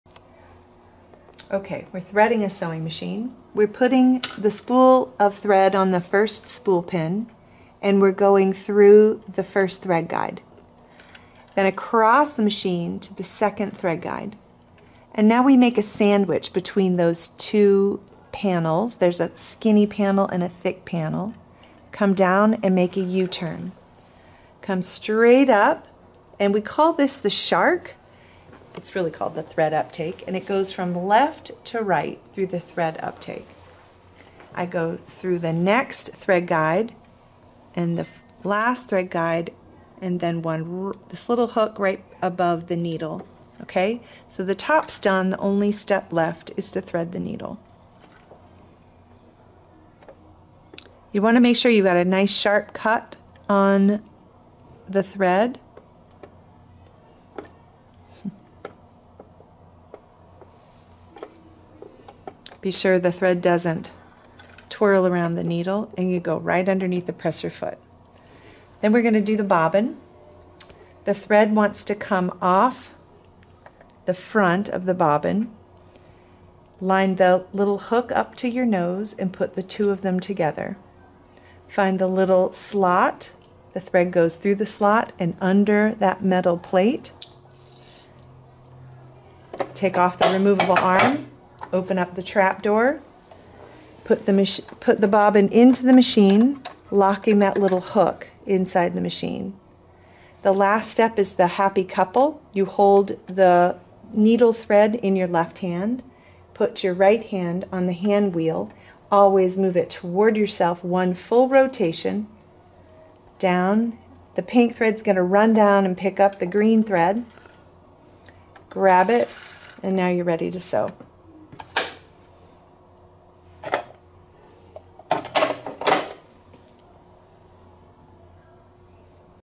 Threading a Sewing Machine Training
Holly demonstrates the proper way to thread one of the sewing machines, at the Amherst Makerspace.